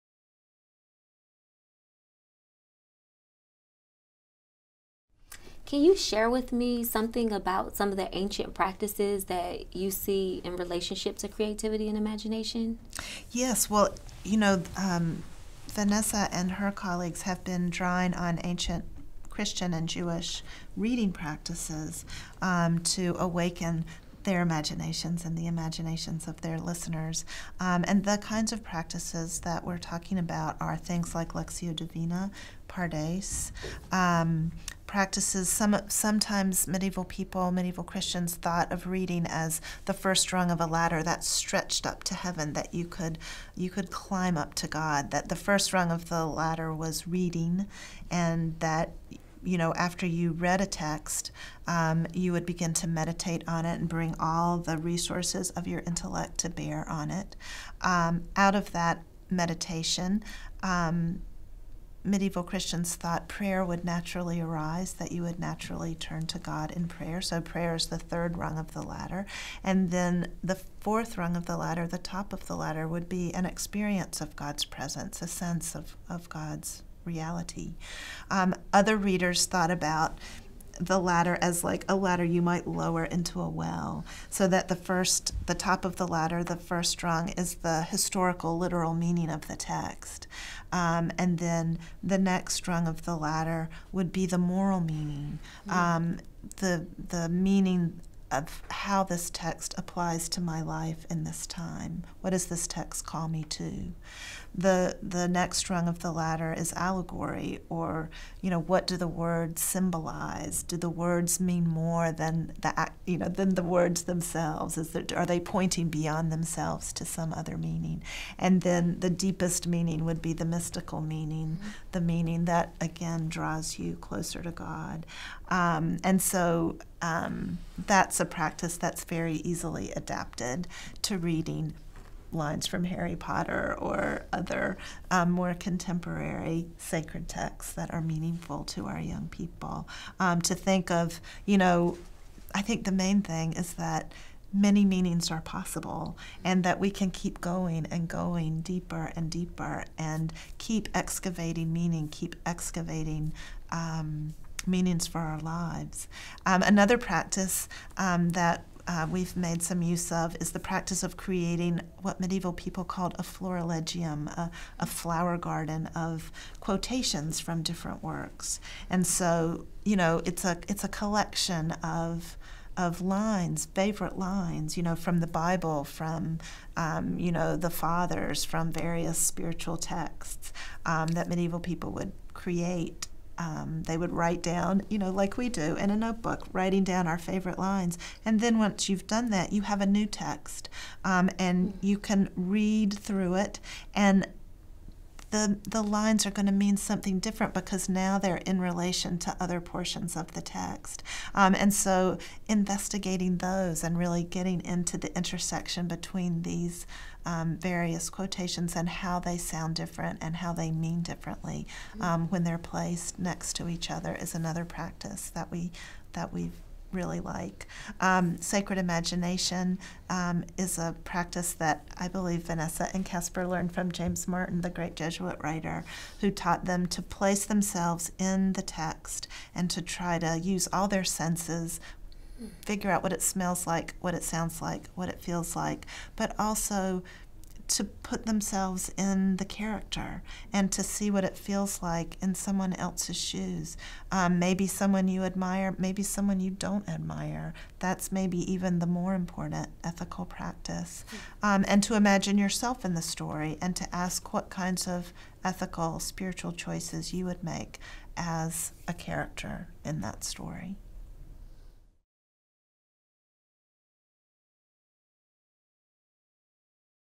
Dr. Stephanie Paulsell and Vanessa Zoltan on Ancient Spiritual Practices & Creativity
Dr. Stephanie Paulsell and Ms. Vanessa Zoltan, MDiv discuss the relationship between ancient spiritual practices and creativity after delivering a lecture entitled, “Matches Struck in the Dark: Imagination and the Joyful Life.”

Dr. Paulsell is the Susan Shallcross Swartz Professor of the Practice of Christian Studies at Harvard Divinity School.

Ms. Zoltan is the co-host of the popular podcast, “Harry Potter and the Sacred Text.”​